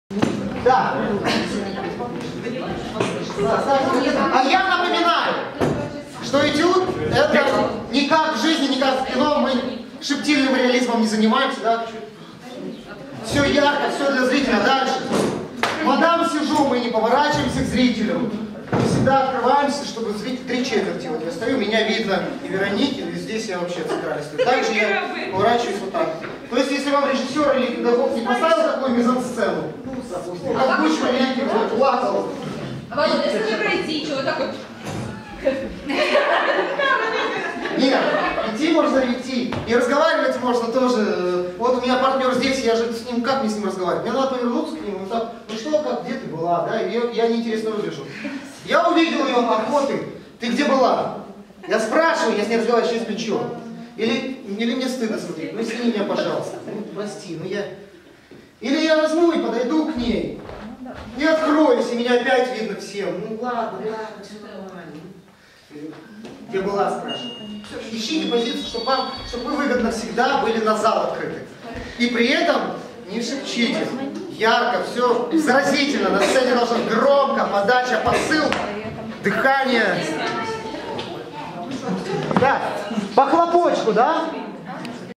Как стоять к зрителю
Владимир Крючков ведет занятие в Школе актерского мастерства ФАРТ. Школа актерского мастерства ФАРТ тел.